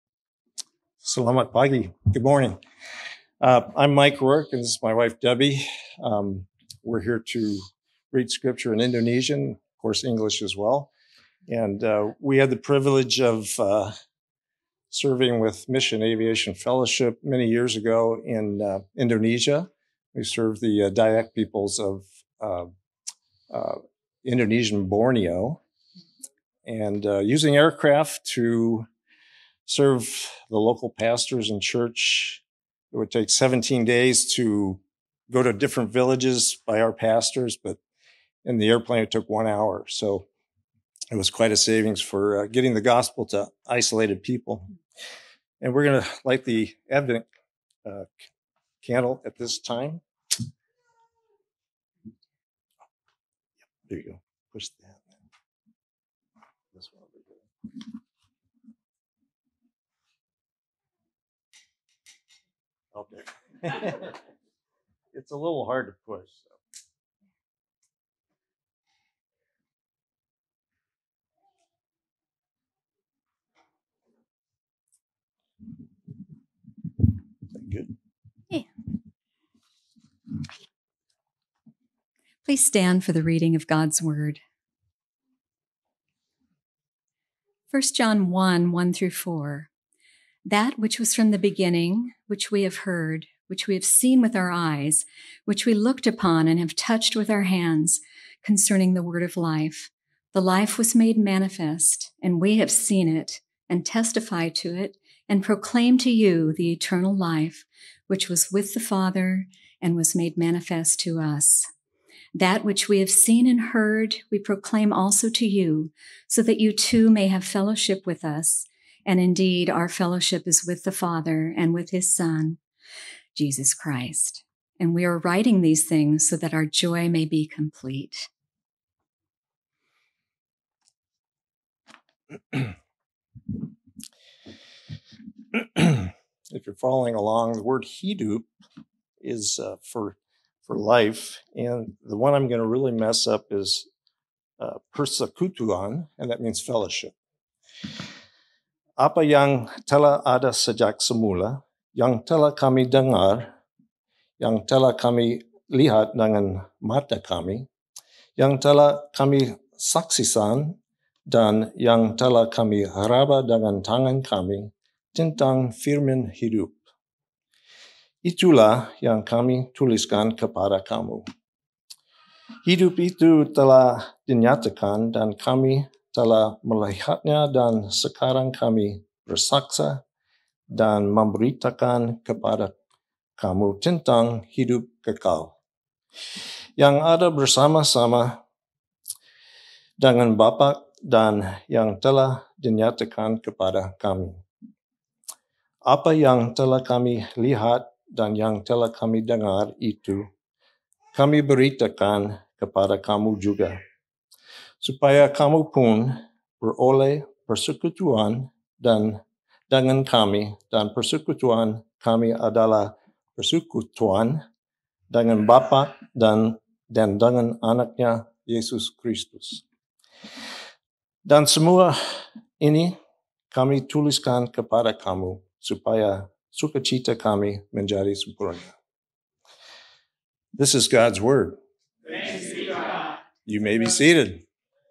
Advent - Week 3 - 1 John 1:1-4 - Spoken in English and Indonesian
Scripture: 1 John 1:1-4
Date: 12/13/2020 11:00am service
Readers: The Roark Family